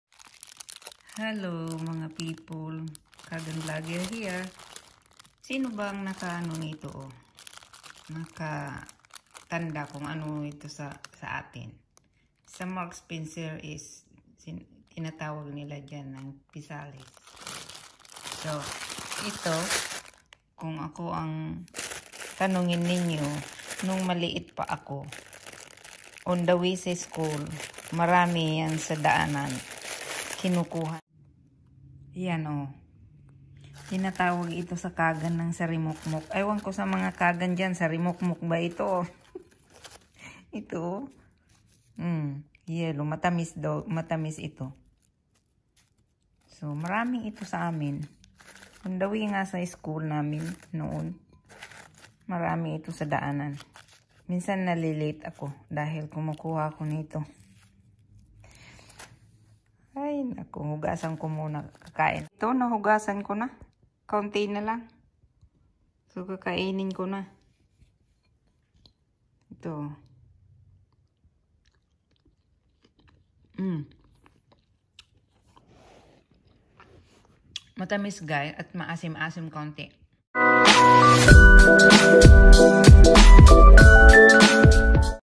How to eat Physalis fruits.
Kaagan Vlogger ❤️
Physalis, this is a small, smooth round fruit wrapped in a papery case that resembles a Chinese lantern. The fruit itself is a pretty orange-gold colour and can be unwrapped and eaten as is, or dipped in melted chocolate because I love chocolate, I dipped it and eat it .